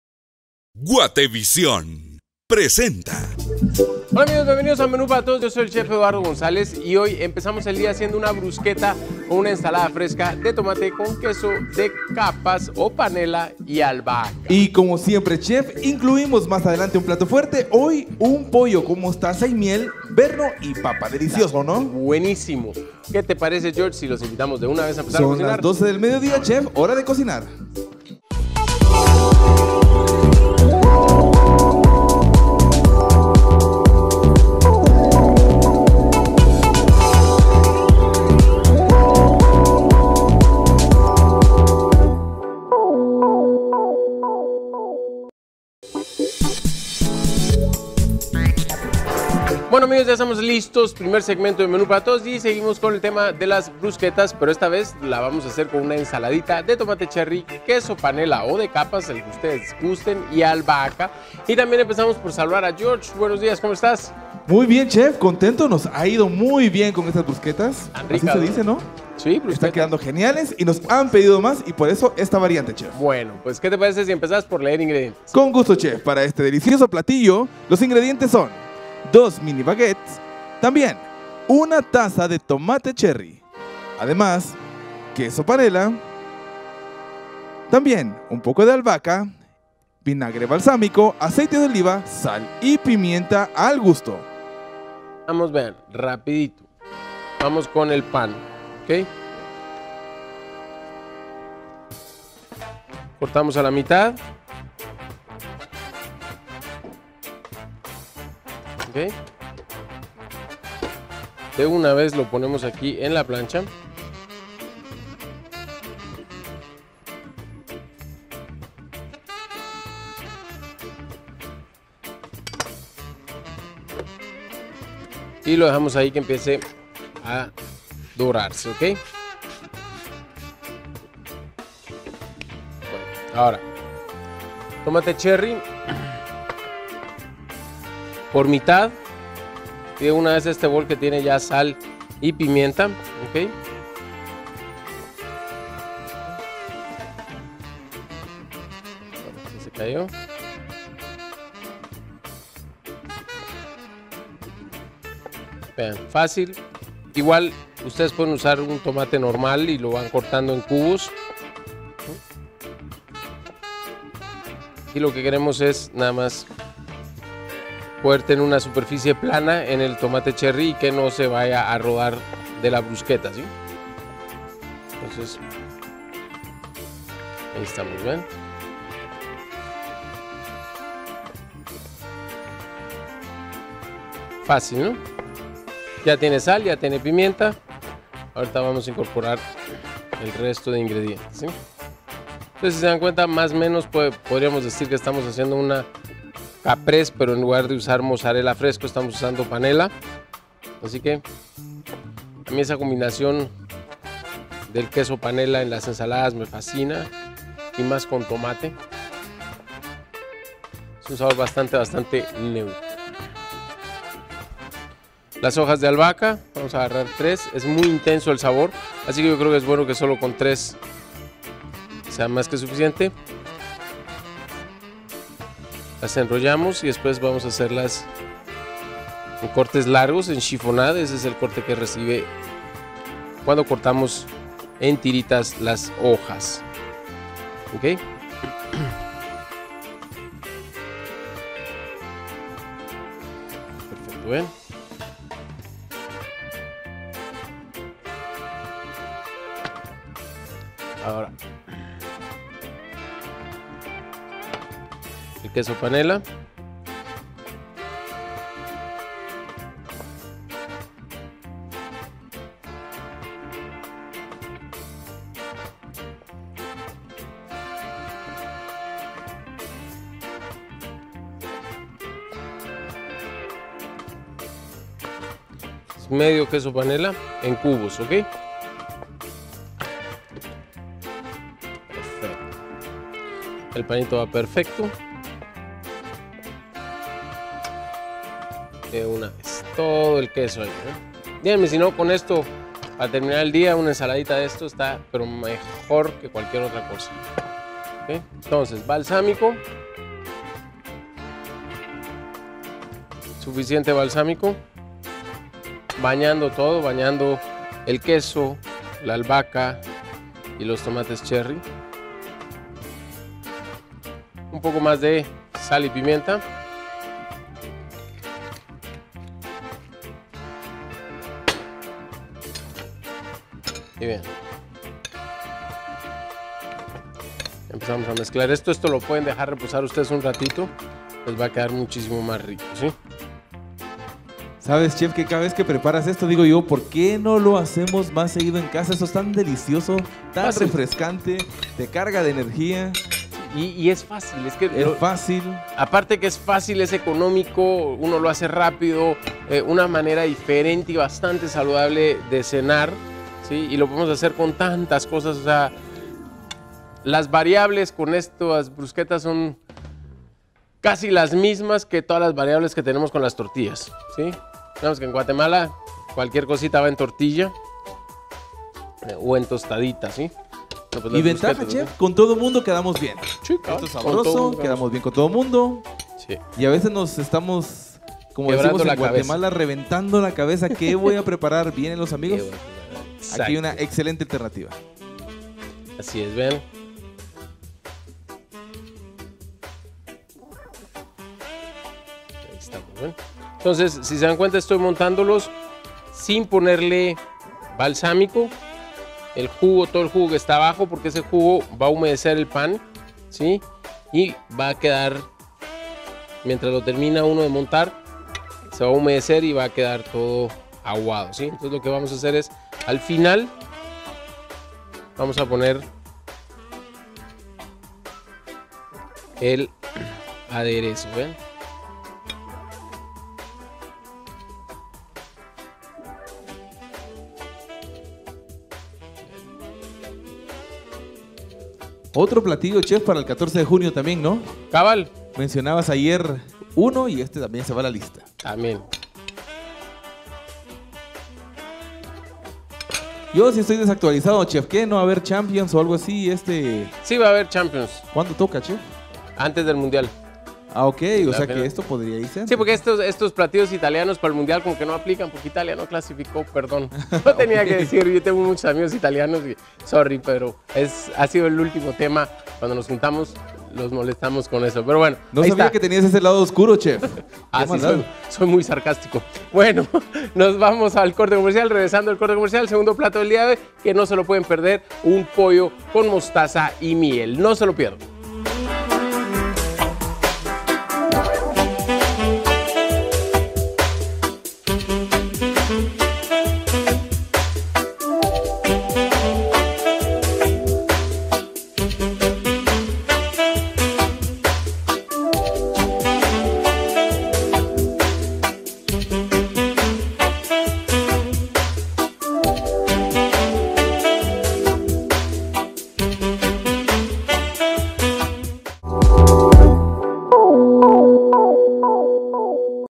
Menú Para Todos: Bruschetta de Tomate Cherry y Queso Panela
Queremos que experimentes con nuevas combinaciones que sin duda provocarán en tu paladar mucha satisfacción.

SIEMPRE VERDE:
Bruschetta de Tomate Cherry y Queso Panela

Ingredientes: 2 mini baquette, 1 tza. de tomate cherry, queso panela, albahaca, vinagre balsámico, aceite de oliva, sal y pimienta al gusto.